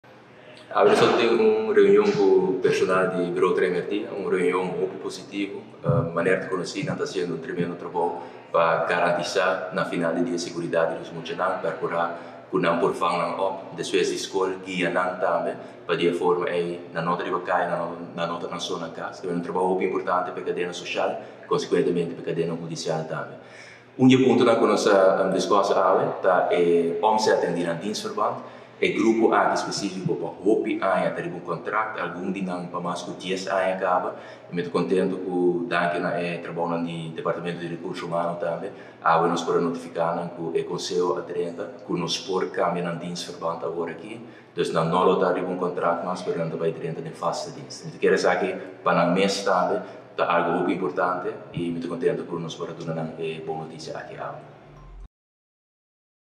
Rocco Tjon: A reuni cu Bureau Trai Merdia
Dialuna mainta, Minister Demisionario di Husticia y Asuntonan Social, Rocco Tjon, a sinta cu e ekipo di Bureau Trai Merdia pa asina comparti cu nan e grato noticia cu despues di alrededor di dies aña, a percura pa esnan cu ta riba un contract por obtene nan "vaste dienst". Banda di esaki, Bureau Trai Merdia ta cumpli 28 aña di existencia, pues tabata un dia di dobel celebracion.